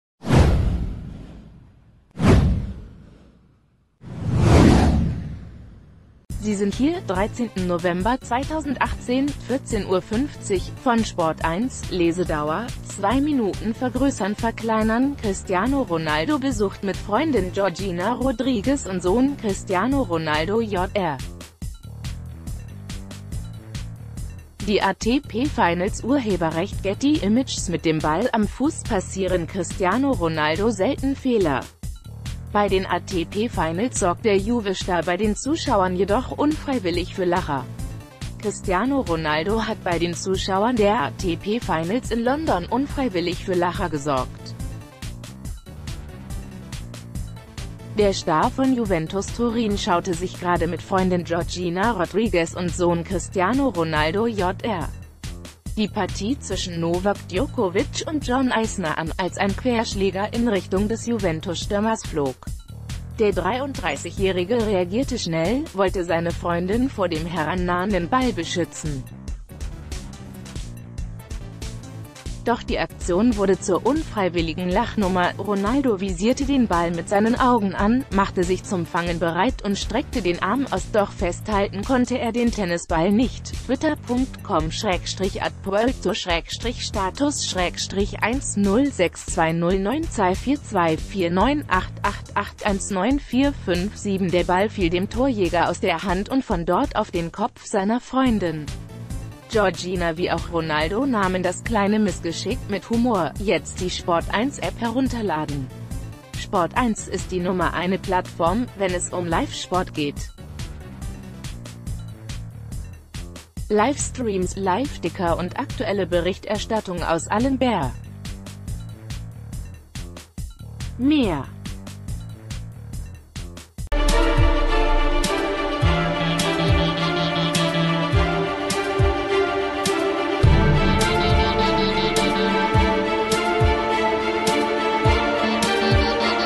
ATP Finals: Cristiano Ronaldo patzt bei Tennis-Parade
Sie sind hier: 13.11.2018 / 14:50 Uhr - von SPORT1 / Lesedauer: 2 Minuten vergrößernverkleinern Cristiano Ronaldo besucht mit Freundin Georgina Rodríguez und Sohn Cristiano Ronaldo Jr. die ATP Finals © Getty Images Mit dem Ball am Fuß passieren Cristiano Ronaldo selten Fehler. Bei den ATP Finals sorgt der Juve-Star bei den Zuschauern jedoch unfreiwillig für Lacher. Cristiano Ronaldo hat bei den Zuschauern der ATP Finals in London unfreiwillig für Lacher gesorgt. Der Star von Juventus Turin schau...